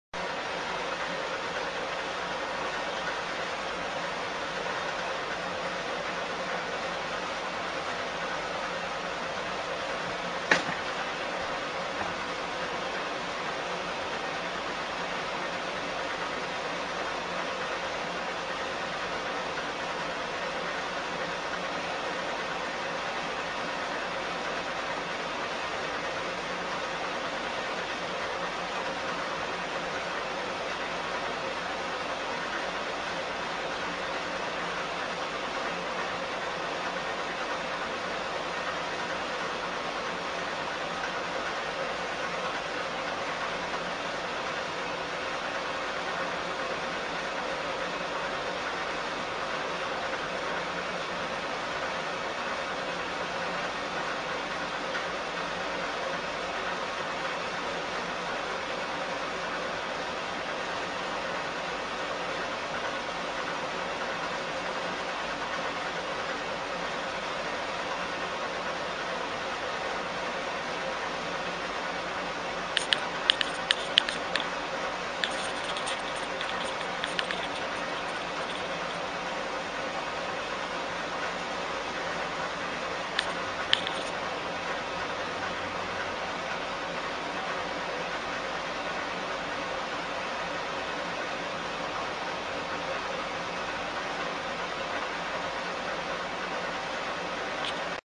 Inside Look at What Goes on at a Typical Freemason Lodge...Ya these guys are a Moral Bunch!
Inside Look @ Temple Prostitute at Freemason Temple